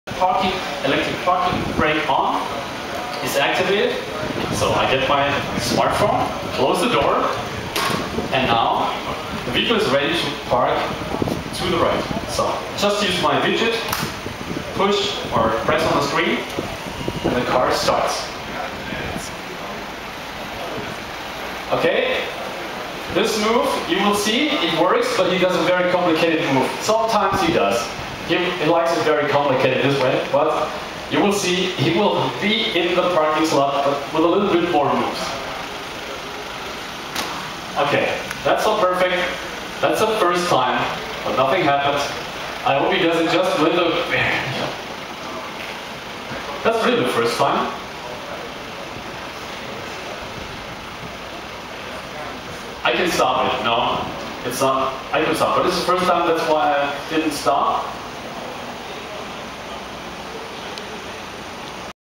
Self driving car bumps into fence, Oops
Demonstration of park4u system goes wrong, car hits the fence...